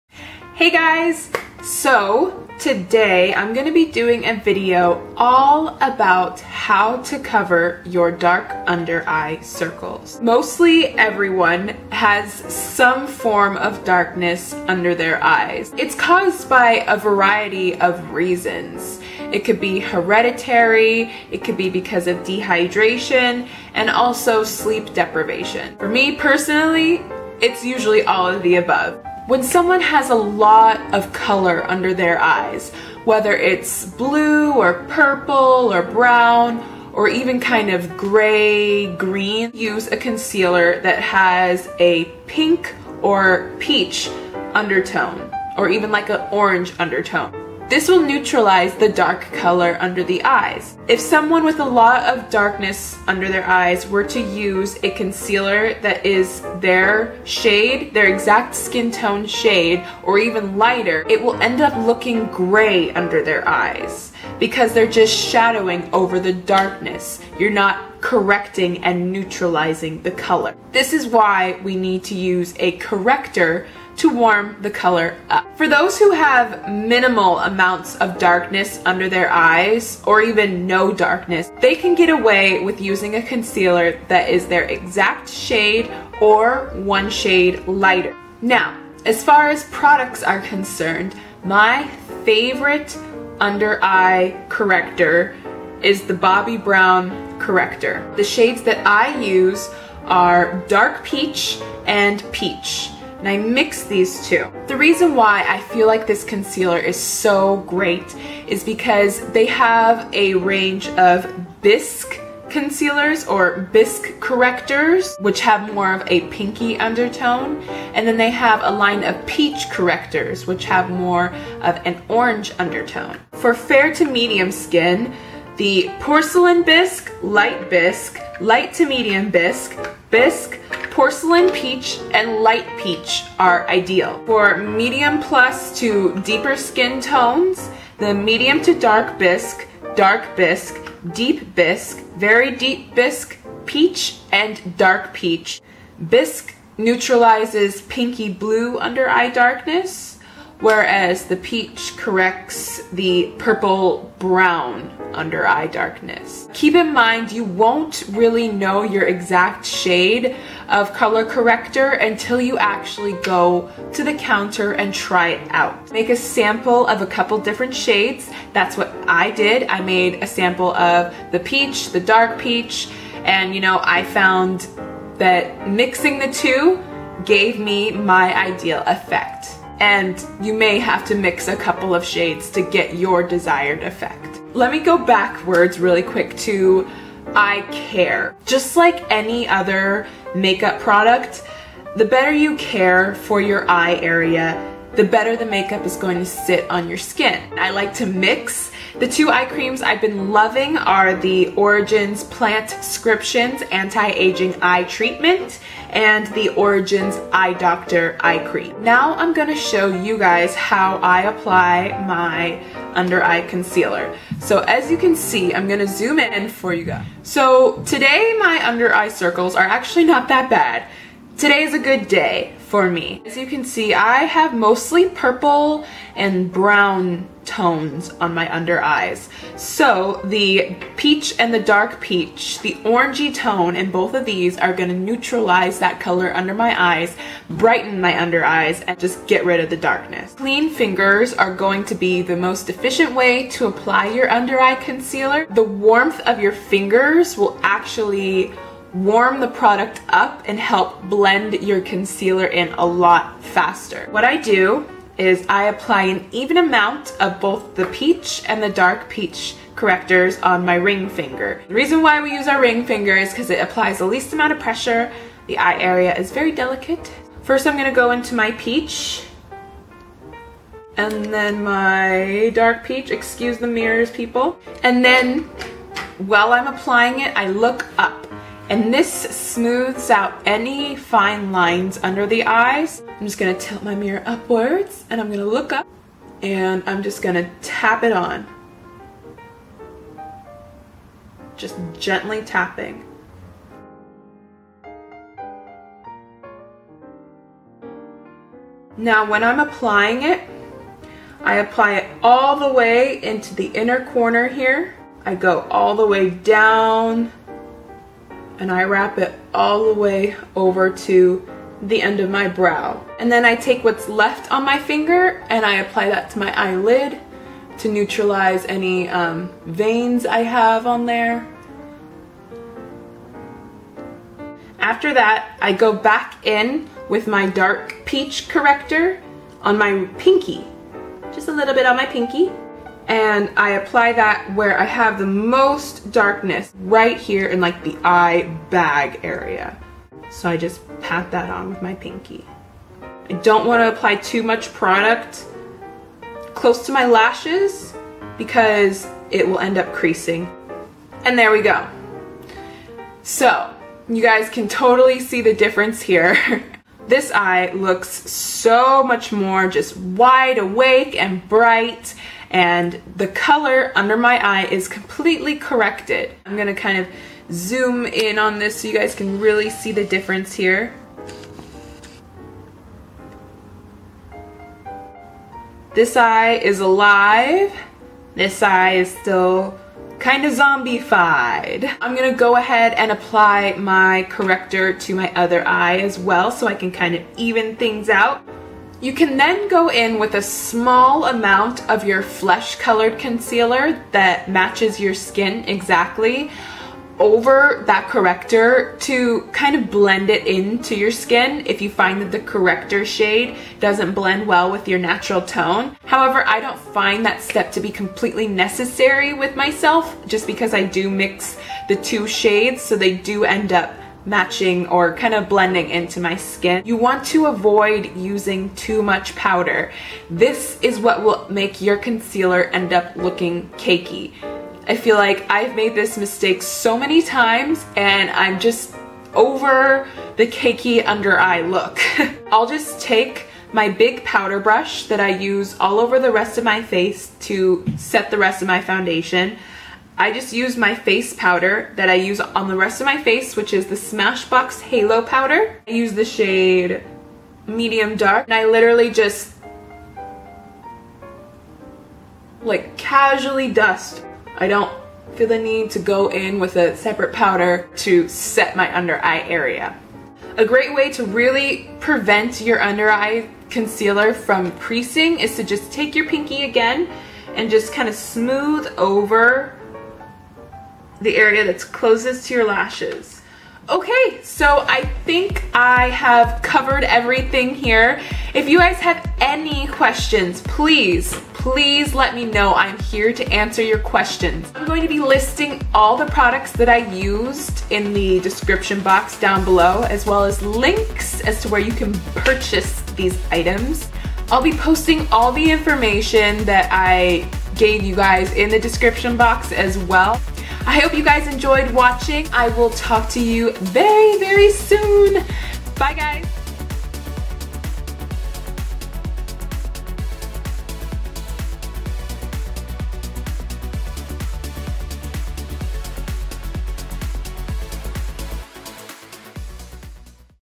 How to Conceal Dark Under Eye Circles
Hey everyone!

Here's all the information:

Theory
Most people have some form of darkness under their eyes. It is caused by a variety of reasons from heredity, to sleep deprivation, and dehydration. When someone has a lot of under eye darkness, whether its blue, purple, or brown, they need to use a concealer that has a pink/peach/orange tone. This will neutralize the dark color under the eyes. If someone with a lot of under eye darkness were to use a flesh colored concealer, their under eye area will end up looking like an awkward light gray color. This is why we warm the area up with a color corrector. For those who have minimal amounts of darkness, using a concealer that is either their exact shade or 1 shade lighter under their eyes is fine.

Products
My favorite concealer for the under eye area is the Bobbi Brown Corrector. The shades that I use are Peach and Dark peach. The reason this concealer is so great is because they have a line of 'Bisque' correctors with a pinky undertone and a line of 'Peach' correctors with more of an orange undertone. For fair to medium skin, the Porcelain Bisque, Light Bisque, Light to Medium Bisque, Bisque, Porcelain Peach and Light Peach are ideal. For medium/plus to deep skin, the Medium to Dark Bisque, Dark Bisque, Deep Bisque, Very Deep Bisque, Peach, and Dark Peach are ideal. Bisque neutralizes pinky-blue under eye darkness. Peach neutralizes purple-brown under eye darkness. Keep in mind that you won't really know your exact shade until you try it out, and you may have to mix a couple shades (like myself) to get your desired effect.

Just like any other makeup product, the better you care for the area, the better the makeup is going to sit on your skin. Using the proper eye cream is super important because it adds extra hydration to the area, as well as working to prevent fine lines, puffiness, and shadows. It also improves the general appearance and feeling of the delicate eye area. The two eye creams I've been loving are the Origins Plantscriptions Anti-Ageing Eye Treatment and the Origins Eye Doctor. I apply both at night, and just the Eye Doctor in the morning. This helps my concealer glide on easily but isn't too greasy.

Application
Your clean fingers are going to be the most efficient way of applying your under eye concealer. I prefer fingers over a brush because the warmth of your fingers will help blend the concealer in to your skin.
When I apply my concealer, I get an even amount of the Peach and Dark Peach shades on my ring finger and pat the concealer on while looking up. Looking up will help smooth out any fine lines under the eyes and allow the concealer to reach the entire area. I then go back in with more of the Dark Peach shade on my pinky and apply that to the darkest area of my under eyes.
You can then go in with a small amount of your flesh colored concealer and pat that over the corrector to blend in the tone with the rest of your face.
I like how the corrector looks by itself, so I do not go back over with a flesh colored concealer.

When you set your concealer you really want to avoid using too much powder because it will cake up. I like to just dust lightly over my eyes with my large powder brush when I'm powdering the rest of my face. This deposits just enough powder so that my concealer won't crease.
Some great powders that are designed for the under eye area are the Ben Nye Luxury Powder, and Laura Mercier Secret Brightening Powder.

What I'm Wearing:
Foundation: Nars Sheer Glow Medium Dark 2 "Tahoe"
Eyebrows: MAC "Brun" & "Coquette" eyeshadow
Mascara: Benefit They're Real
Lips: Nars Velvet Matte Lip Pencil "Dragon Girl"